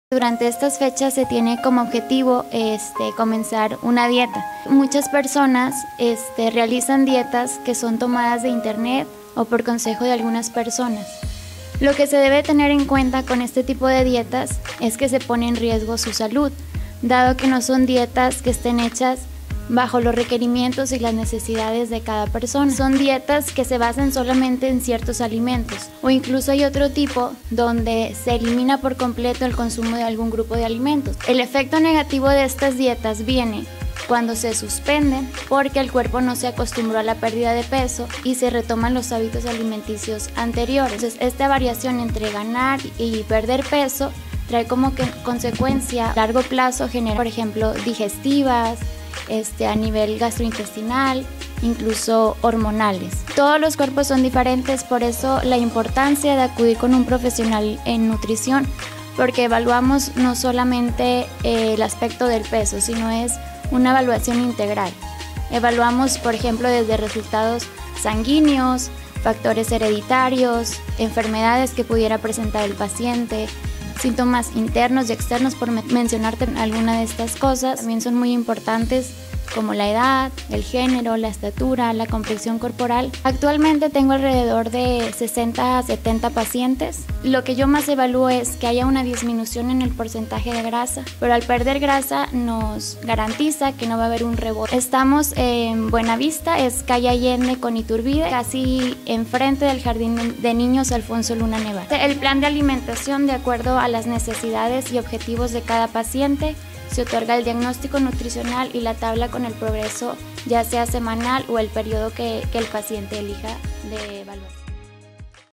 ¿SABES LOS RIESGOS DE LA AUTO DIETA?
Allende.- Como fin de año, miles de personas les interesa empezar el año con alguna dieta balanceada más sin embargo muchos de los errores es hacer una auto dieta a través de internet, o el simple hecho de algunas recomendaciones por amigos.
La Nutrióloga; Mayra Alejandra Martínez Hernández, nos da algunas recomendaciones del por qué no usar una auto dieta y los problemas que pueden causar; principalmente un consejo de asistir con Nutriólogos de Profesión, su misión como parte de su trabajo es ayudar a las personas a disminuir la grasa corporal con dietas que la persona necesite ya que no todos tienen un cuerpo igual y que no den el llamado “rebote”.
Con alrededor de 70 pacientes cuenta los cuales algunos iniciaron desde el mes de Diciembre, con excelentes resultados, se encuentra ubicada en Calle Allende con Iturbide, Buena Vista.